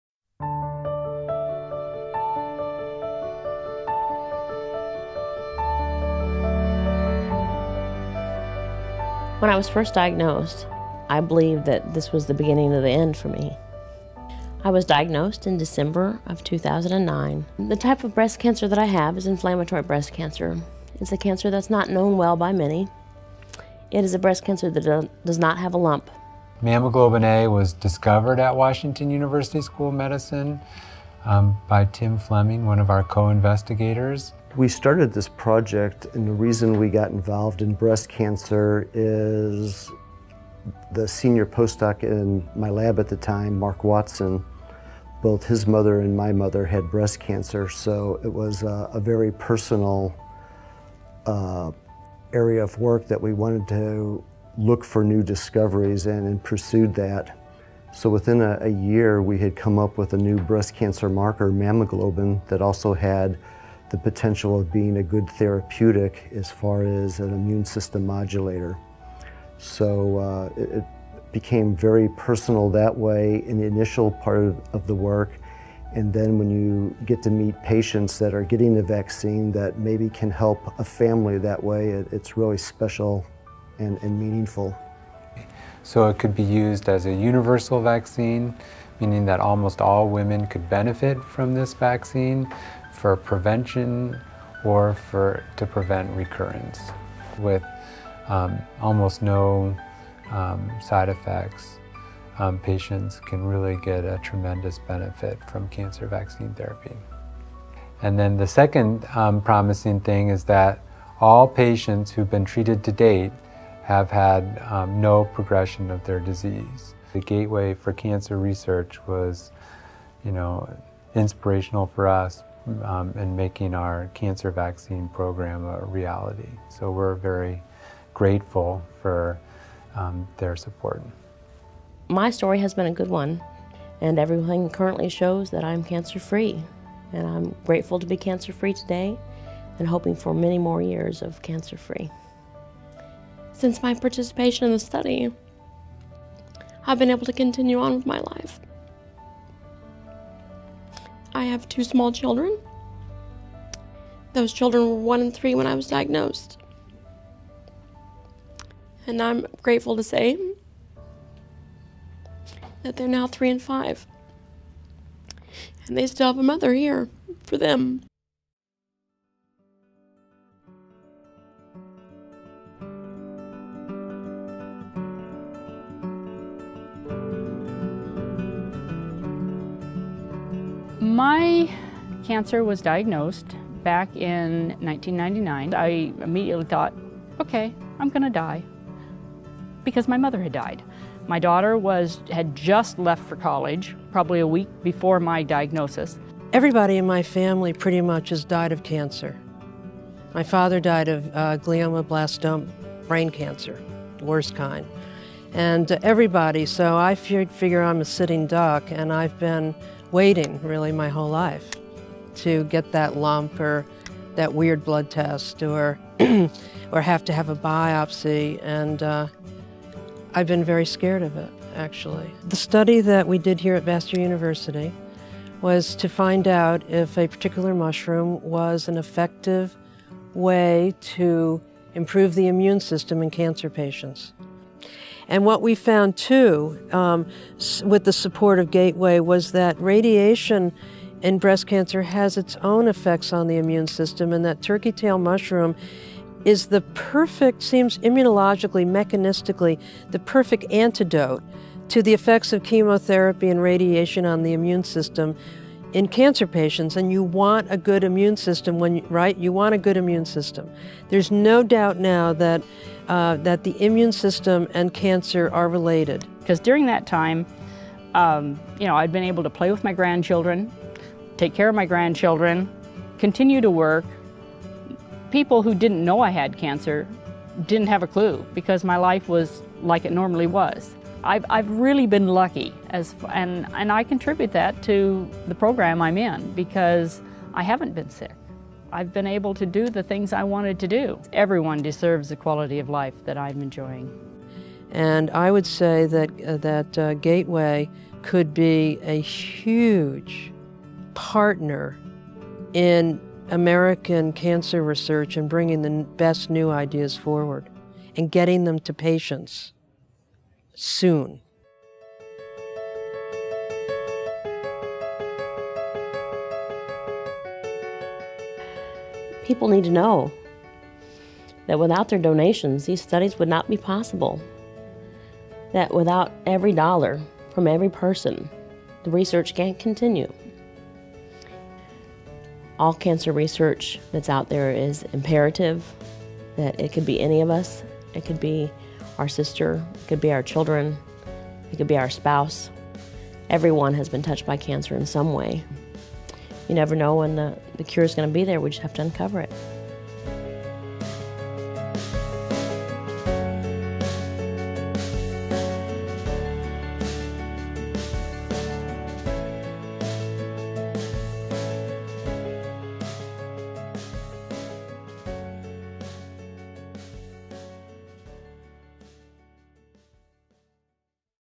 Inflammatory Breast Cancer Vaccine: Treatments & Cures
Cancer survivors and cancer researches talk about how the Gateway for Cancer Research has helped fund studies that will improve the lives of cancer patients everwhere right now.